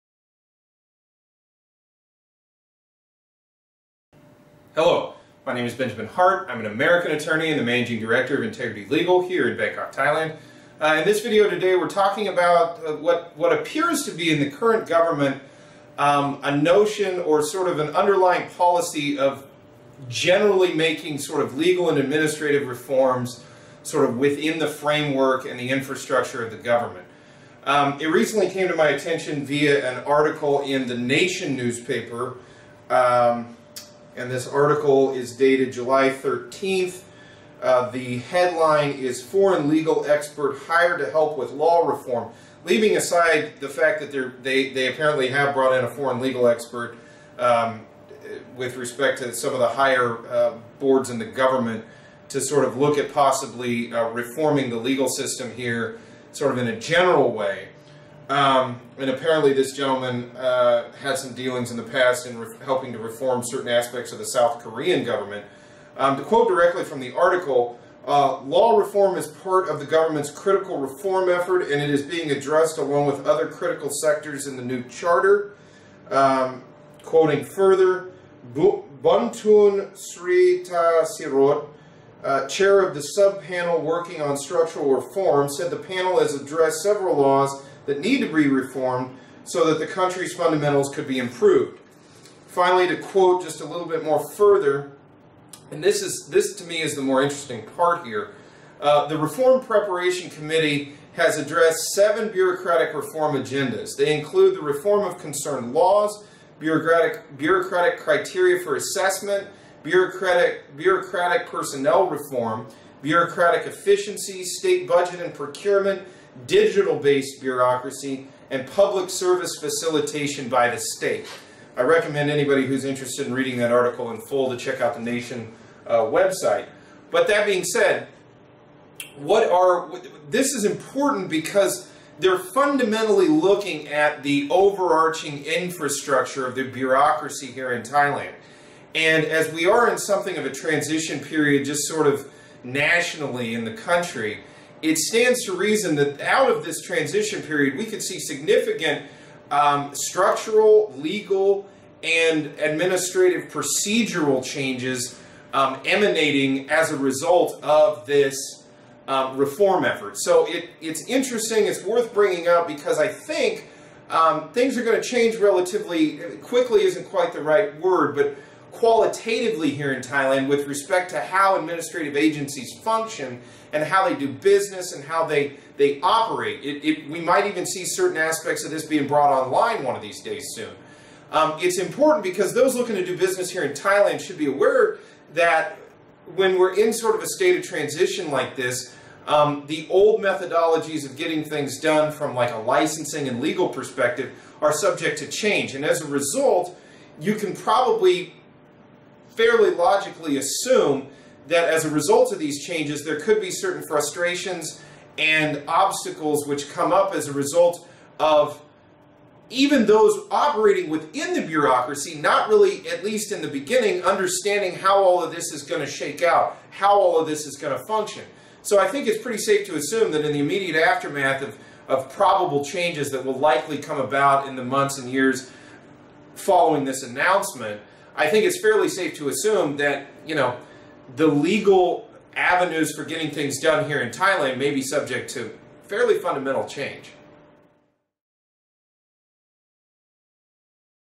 Thailand Considering Some Legal and Administrative Reforms
It appears as though officials in Thailand are seriously considering certain types of reform.